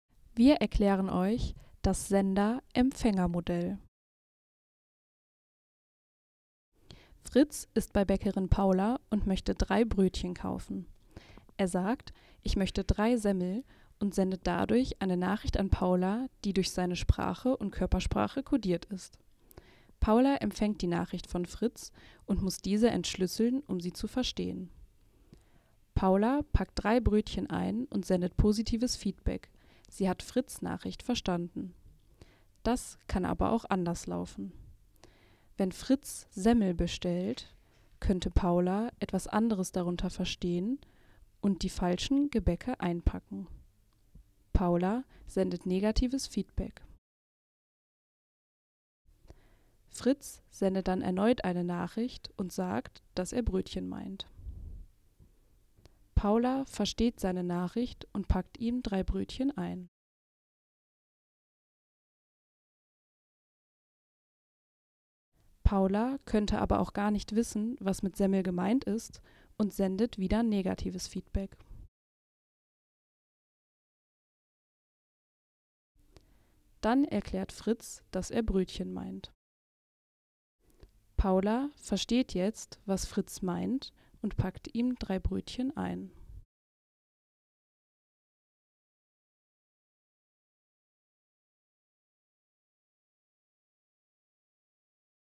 Das Sender-Empfänger-Modell
Wir erklären euch hier das Sender-Empfänger-Modell nach Paul Watzlawick. Schreibt uns gerne euer Feedback in die Kommentare!